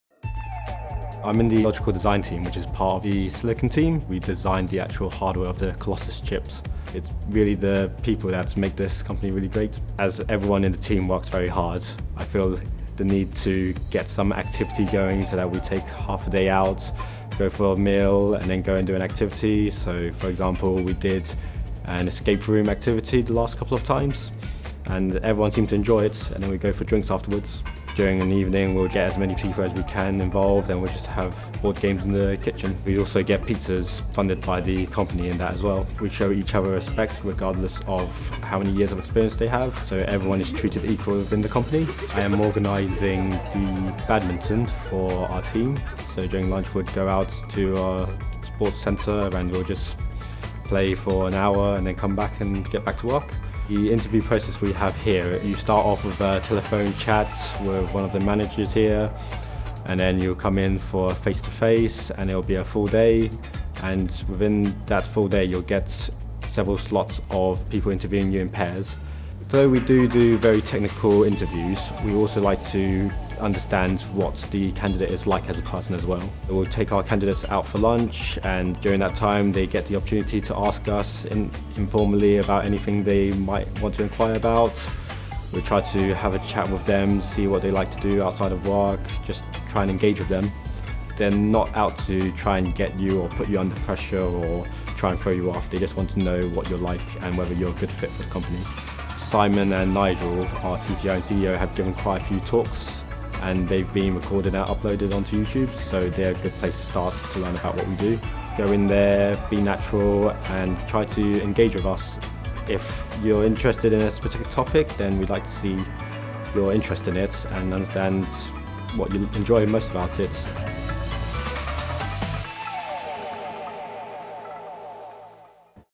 #8 Working at Graphcore - Silicon Engineering
What's it like to work at AI chip company, Graphcore? Meet Samuel, one of our silicon engineers in Bristol, to find out more. If you're coming for an interview  or thinking about applying for a job, then check out our videos to find out more about our team, our culture and what to expect.